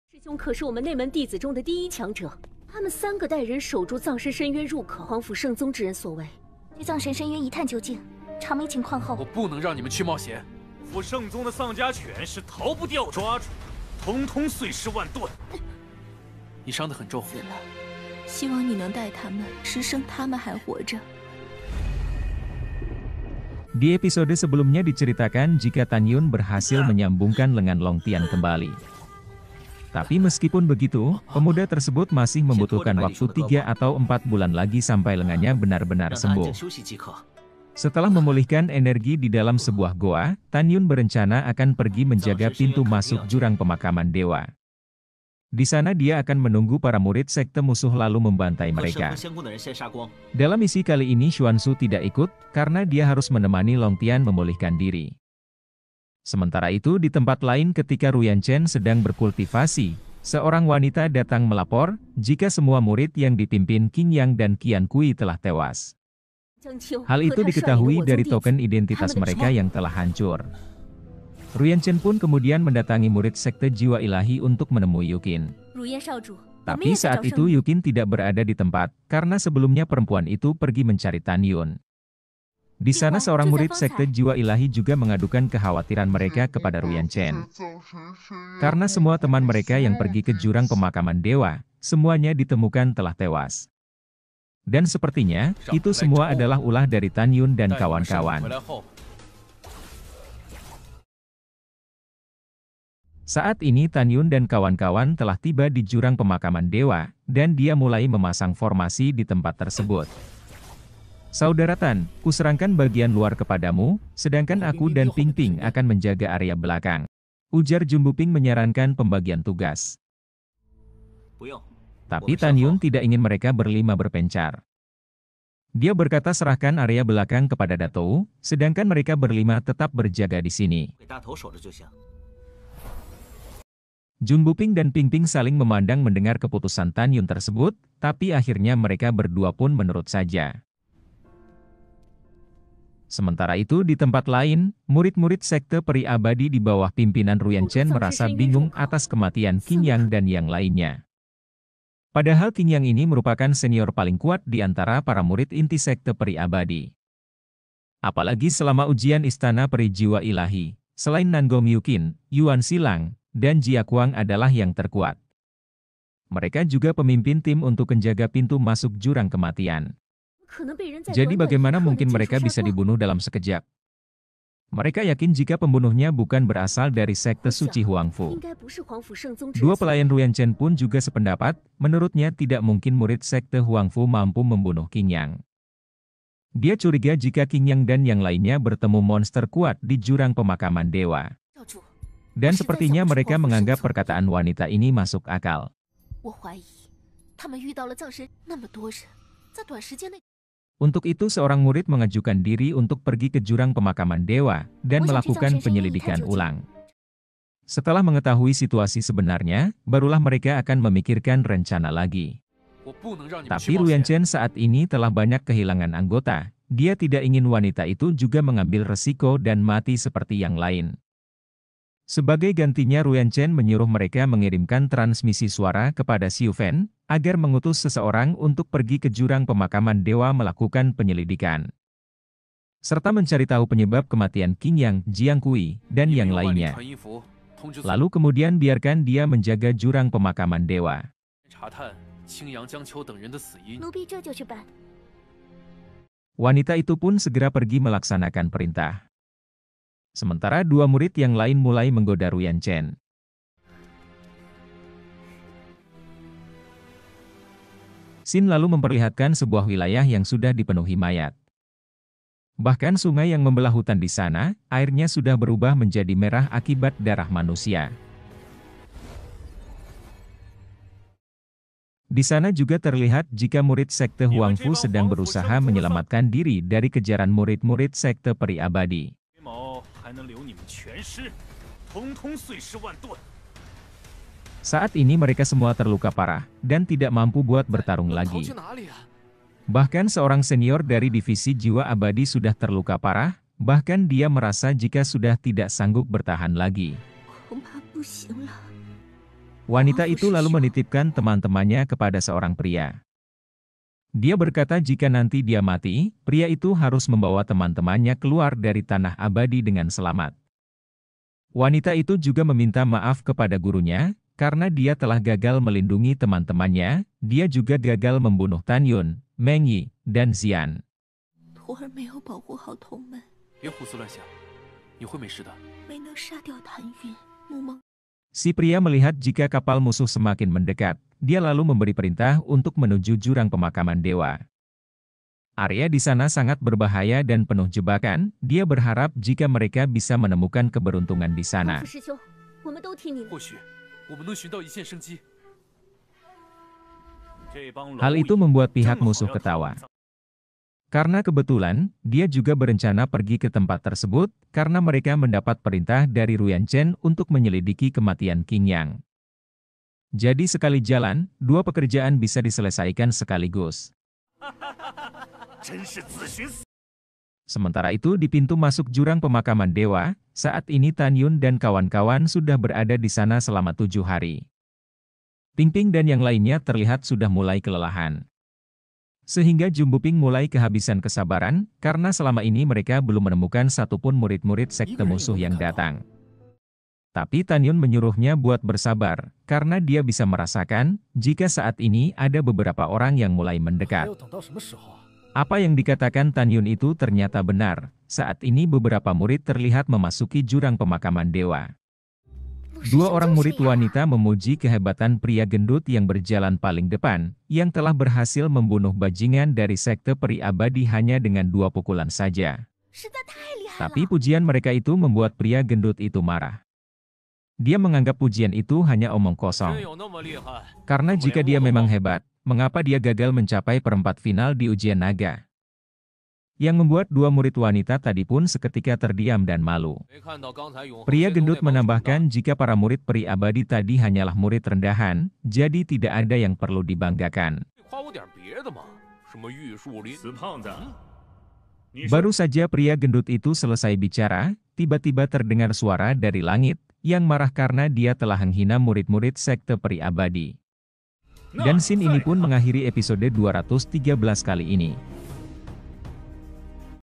TanYun Mulai Menjalankan Misi Pembantaian
Dewa PakBoy Membantai Semua Murid Sekte Musuh

Donghua, Donghua Sub Indo
Film Donghua Cina